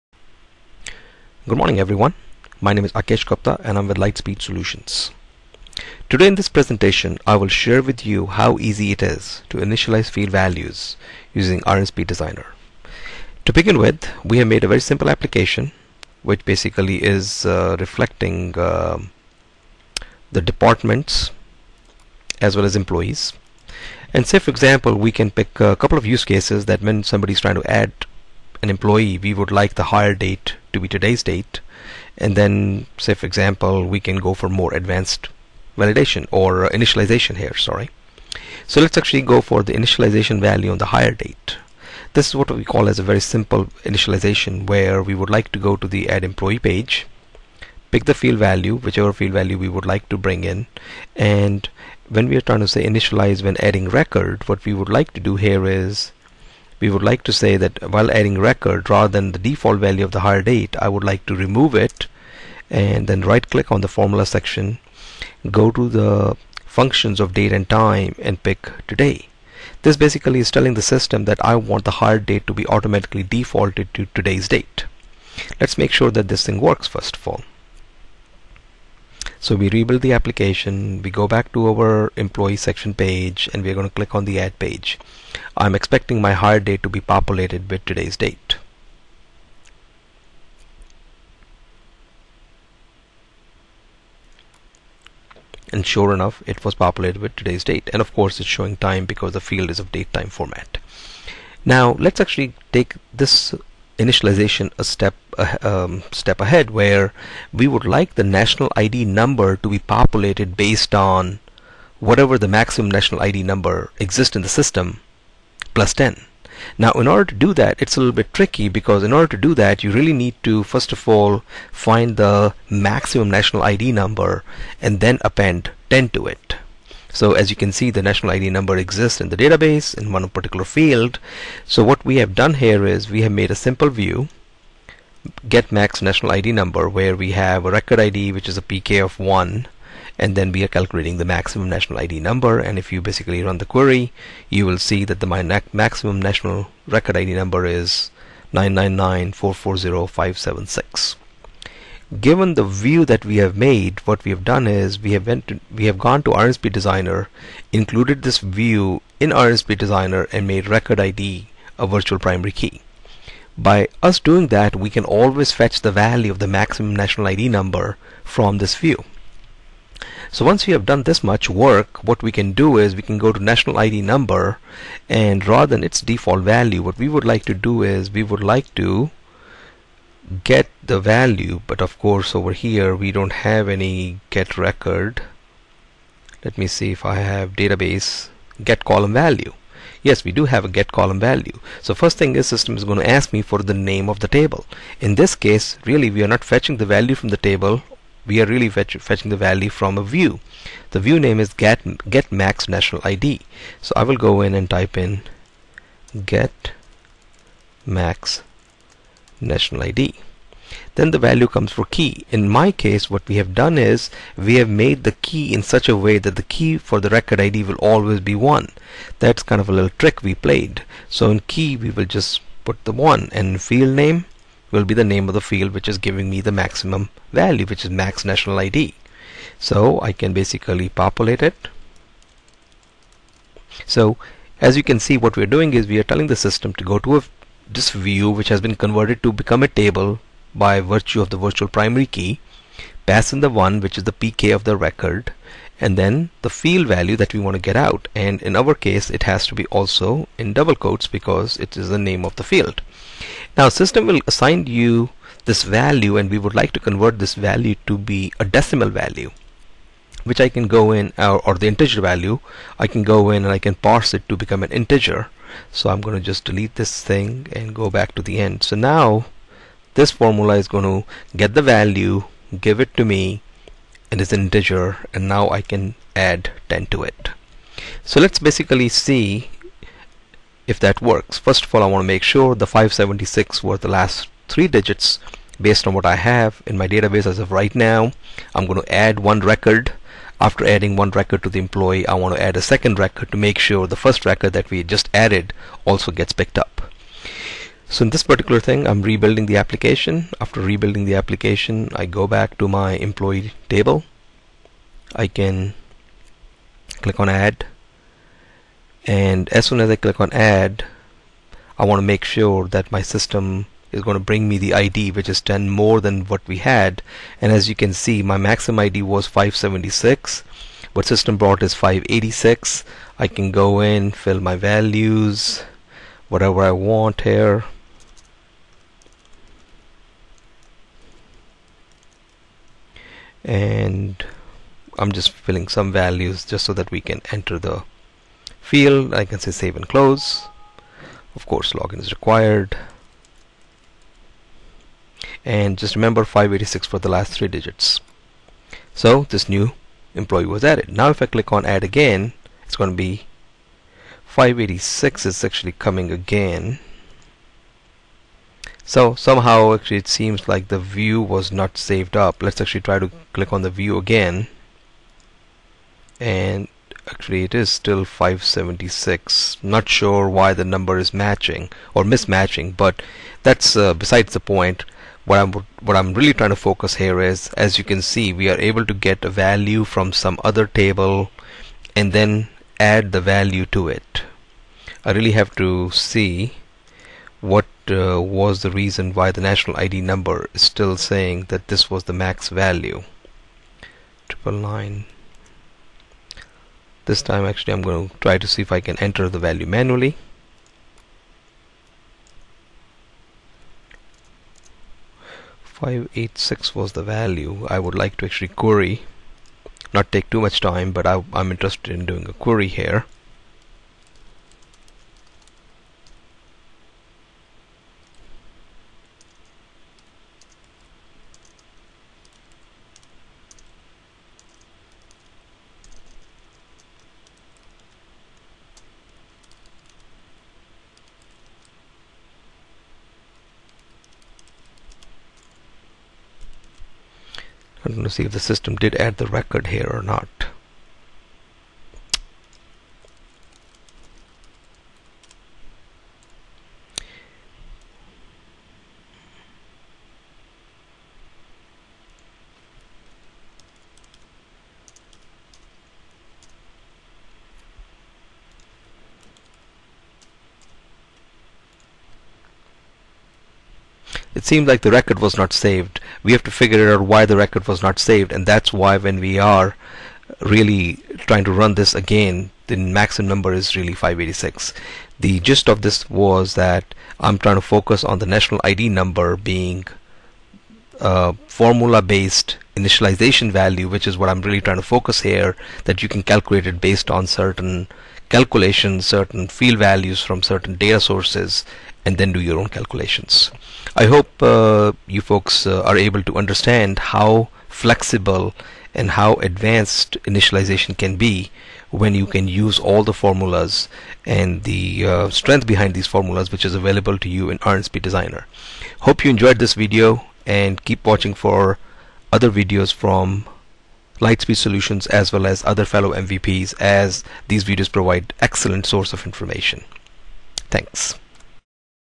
Initializing Field Values with Formulas - Iron Speed Designer V7.1
Akesh Gupta of Light Speed Solutions demonstrates how to initialize a field value using an Iron Speed Designer formula.